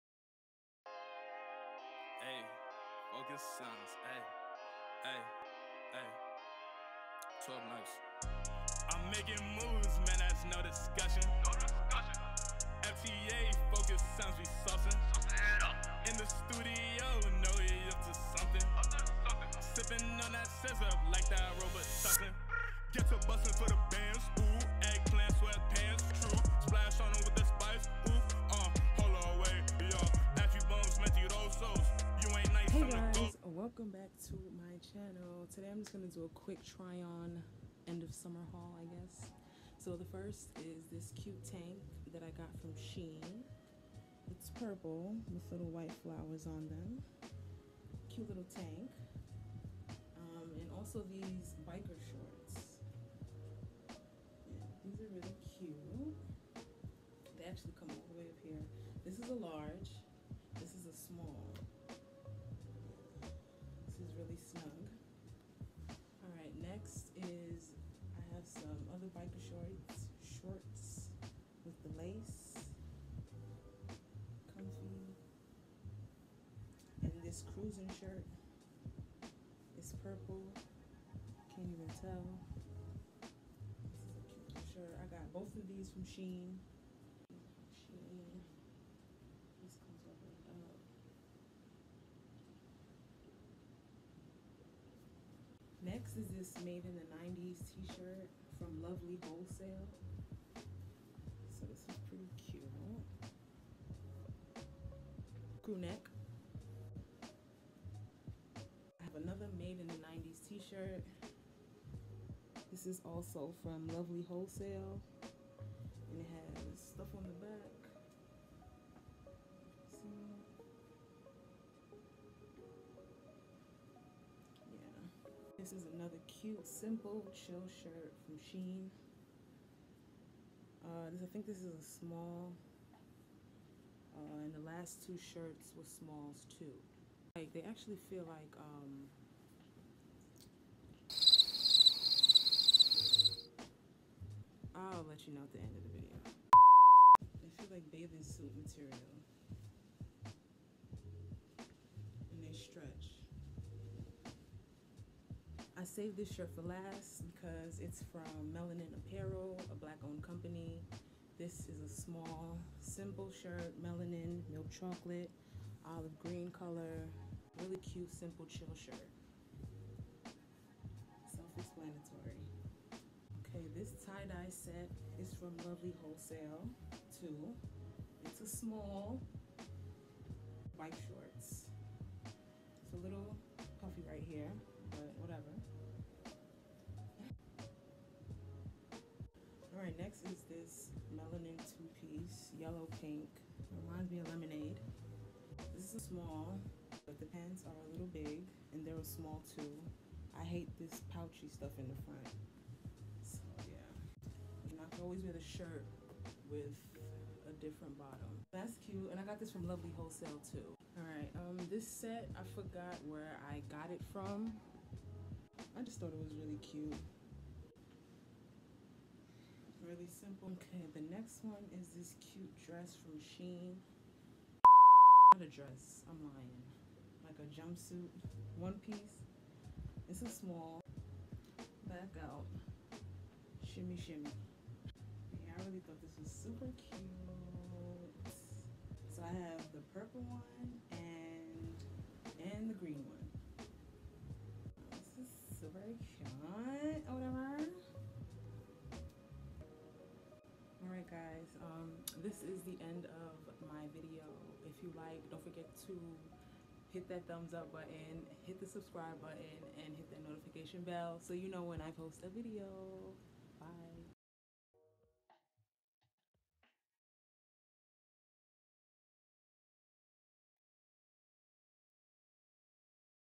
QUICK ! T-SHIRT TRY ON HAUL👚 | ARTSYREB3L IMANI
HEY GUYS! Just a quick try on haul before the weather starts changing. I was so excited for my Melanin Chocolate shirt.